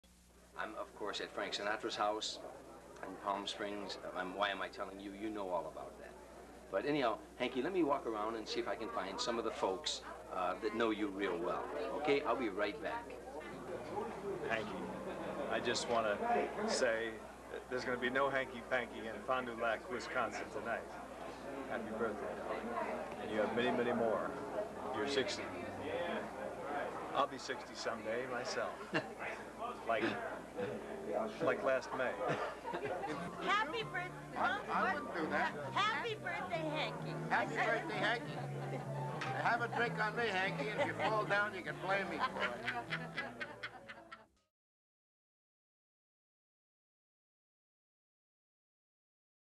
Johnny's mom Hanky gets birthday wishes from Frank Sinatra, Liza Minnelli & Clint Eastwood!
Tom Dreesen takes a video camera to Frank Sinatra's Palm Springs home to get some of the guests to wish Johnny's mother a happy 60th birthday -1991!